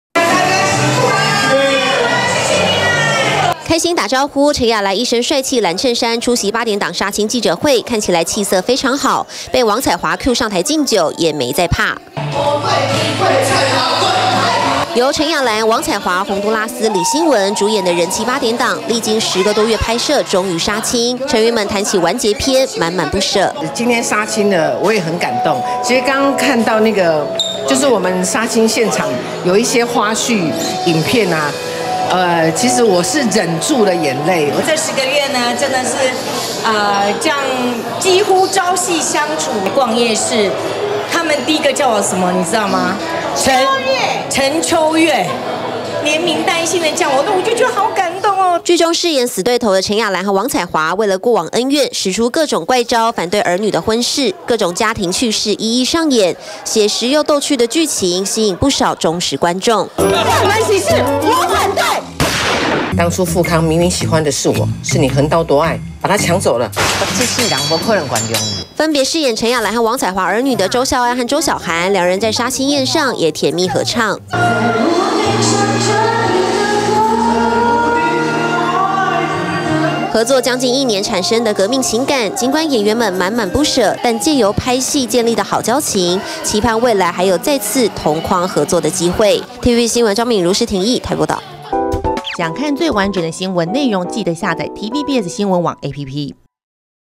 《加油喜事》殺青了! 陳亞蘭.王彩樺喊不捨 周曉涵.周孝安甜蜜合唱｜TVBS娛樂頭條
陳亞蘭、王彩樺、洪都拉斯、李興文主演的八點檔，歷經10個月拍攝，終於殺青了。劇中飾演死對頭的陳亞蘭和王彩樺，戲外展現好友情，在殺青宴上同台高歌。陳亞蘭甚至還差點淚灑現場，直言真的很不捨。